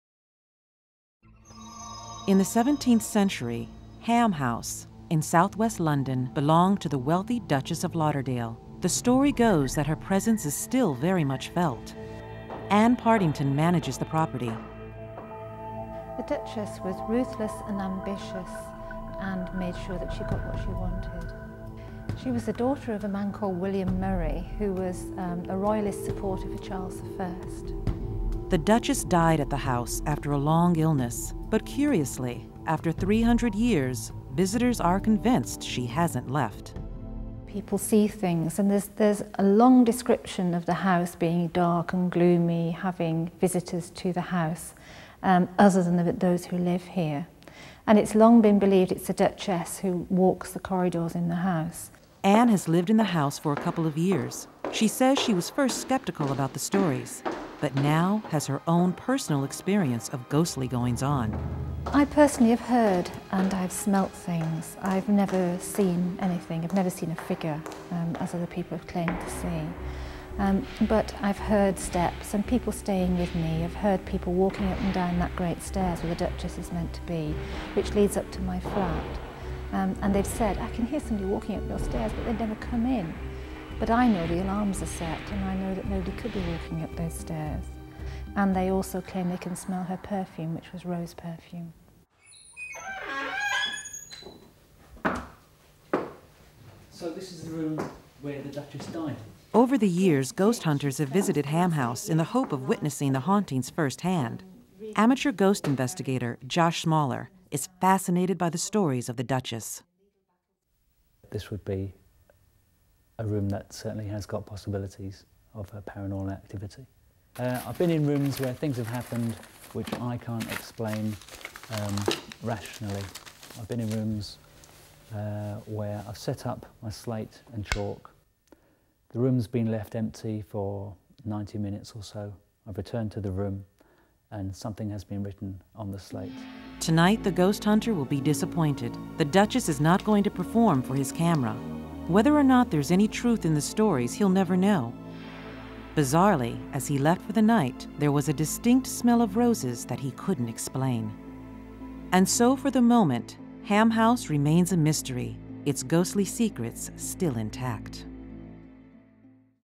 Haunted Ham House | National Geographic
A large mansion in London might be haunted by a woman who's used to getting her way.